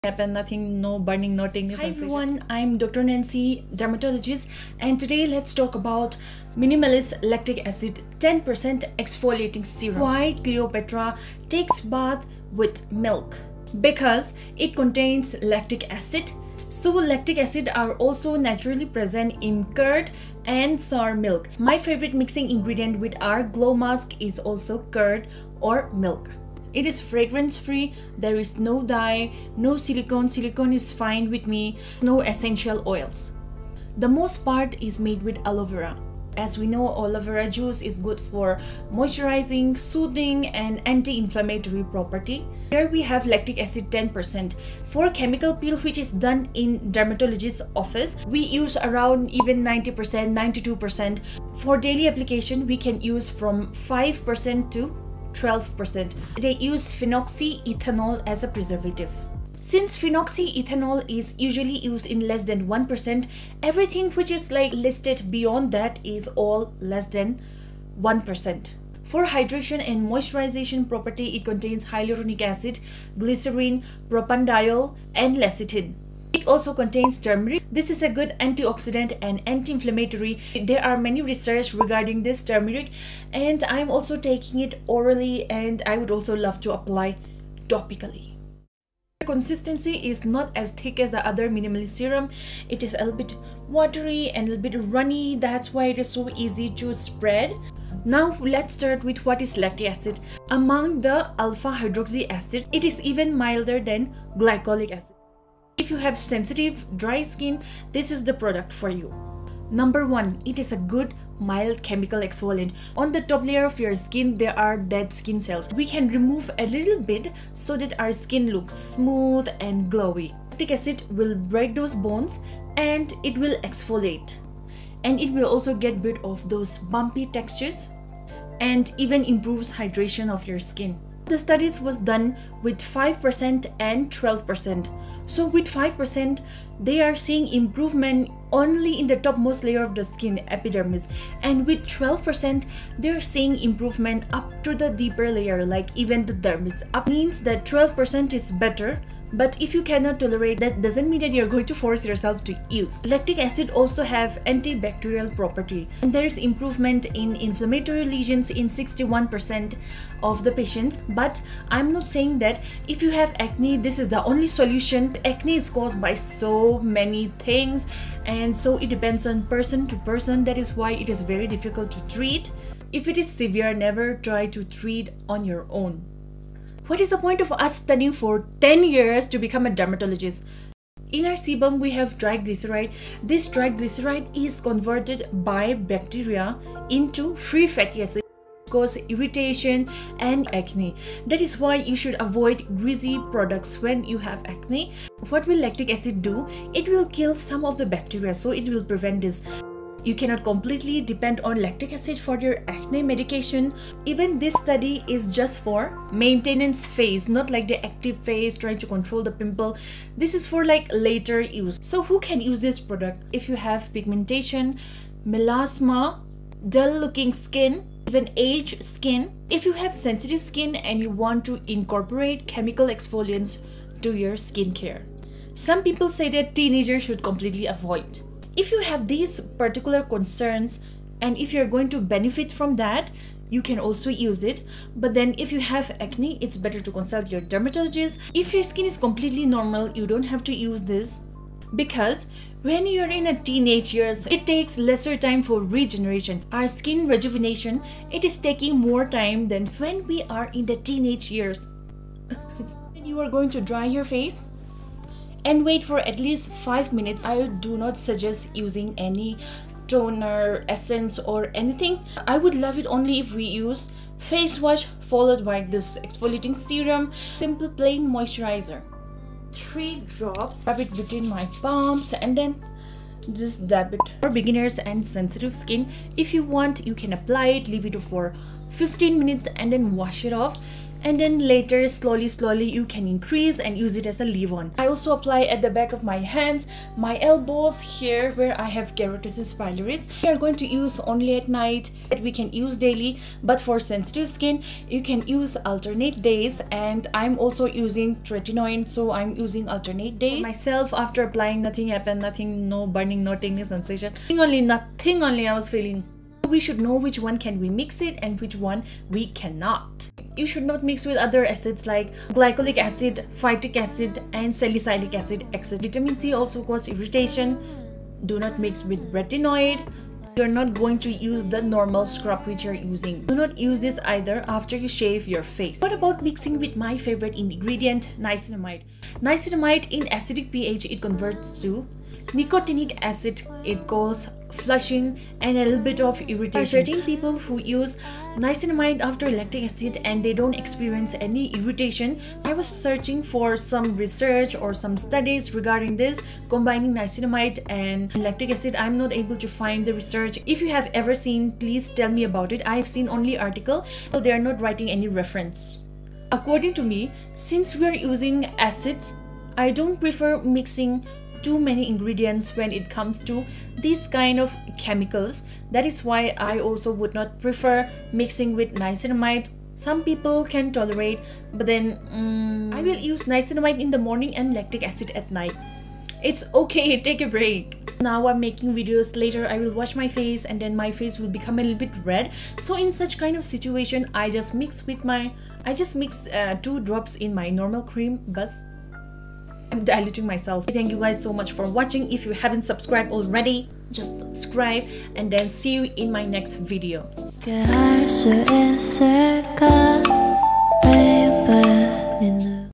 Minimalist lactic acid serum 10% review after 2 months| Dr nancy love skin
Best serum for sensitive and dry skin with pigmented skin!
It is completely safe to use. For me it does not cause any tingling or burning sensation.
I use it every alternate nights, in between i use my retinoids.
For beginners: after 15 mins rinse with water and apply moisturizer, later slowy use it as a leave on.
All studies done was used as day and night application of 5-12% so yoy dont have to worry!
It gently exfoliates give glow and hydration to your face.
* Good for sensitive, dry skin, dull, ageing skin, pigmentation.
* Not that effective for active mild to severe acne.
MINIMALIST LACTIC ACID 10% SERUM
1) FACE WASH (NORMAL NONMEDICATED)
EPISOFT FACEWASH

2) SALICYLIC ACID FACEWASH: (MORNING: OILY SKIN)
SASLIC DS

3) MINIMALIST NIACINAMIDE
(with lactic acid use in the morning, lactic acid at night)

4) VIT C SERUM: (Morning)
MINIMALIST VIT C

5) ACNE OC GEL MOISTURIZER

6) SUNSCREEN:
RIVELLA GEL